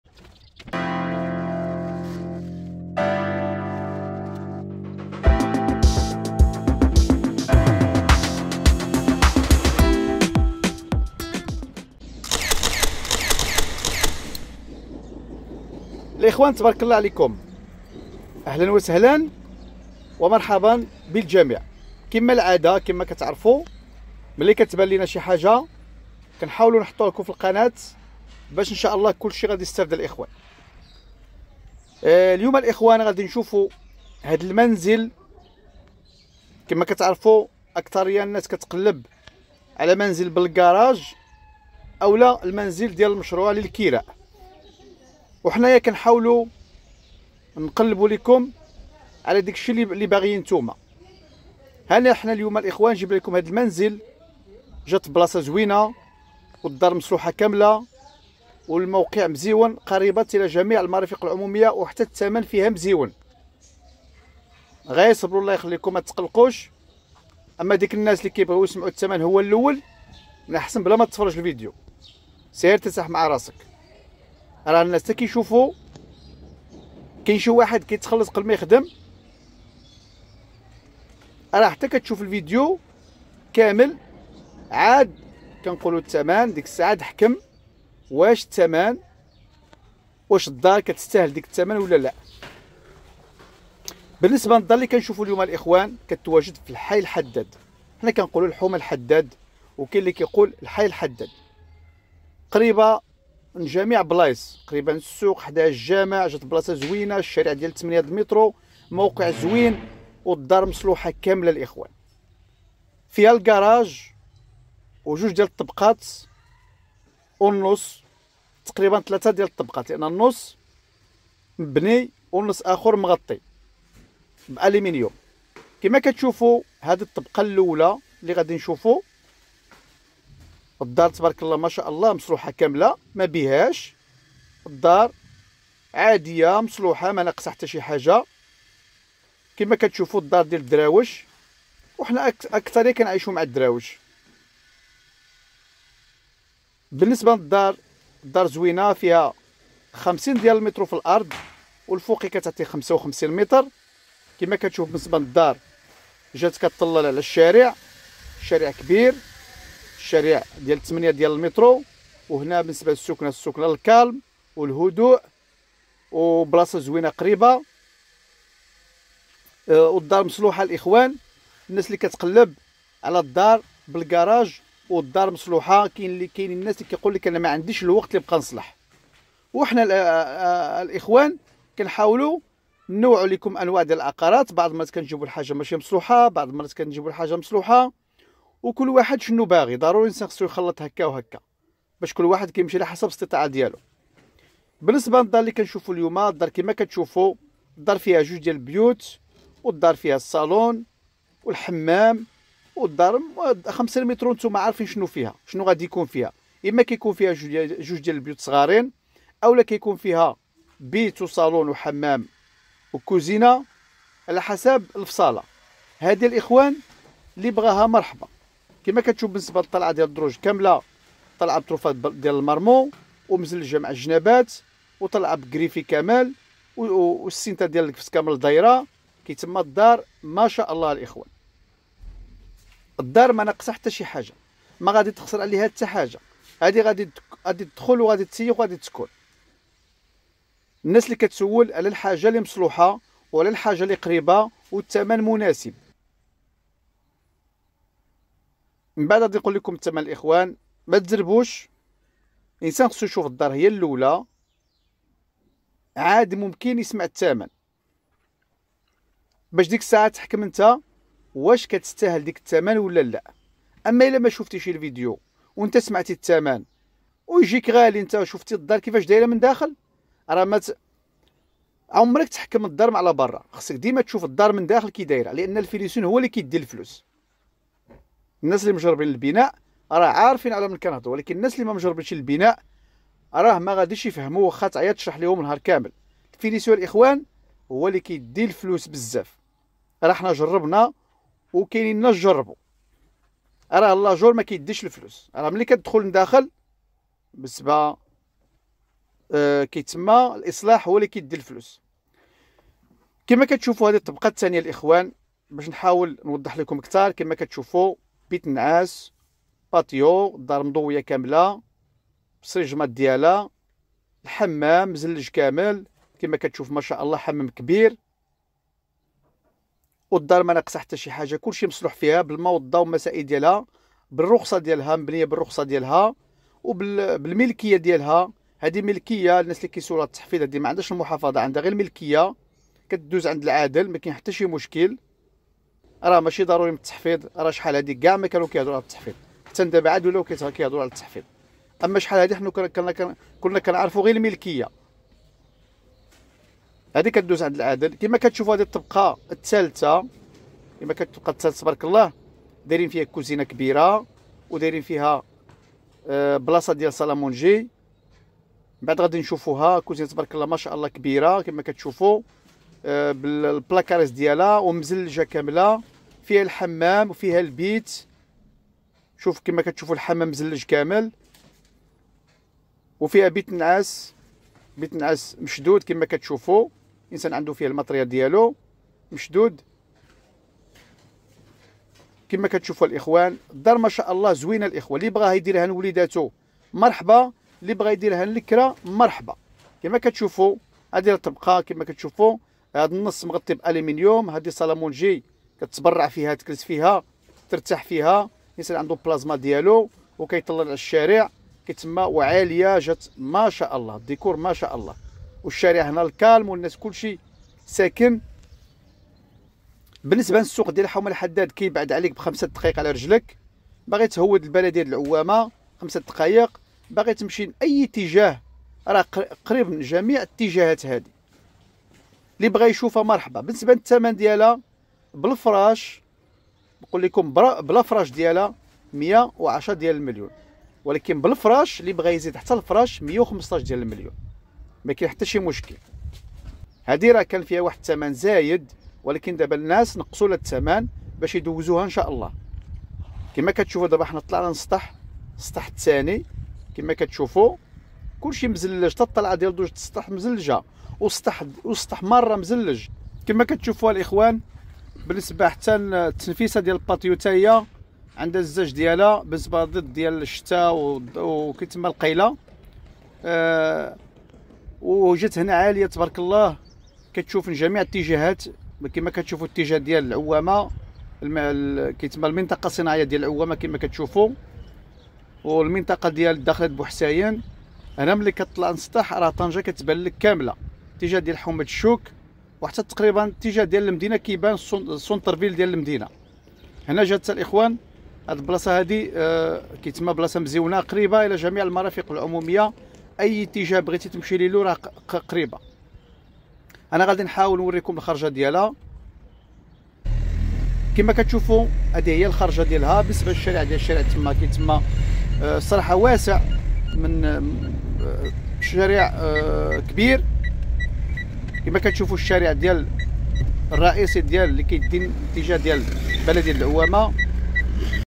منزل روعة للبيع 3 طوابق وكراج حومة الحداد طنجة تمن في متناول مرحبا بالجميع
منزل للبيع طنجة منطقة العوامة 115 مليون مع المفاهمة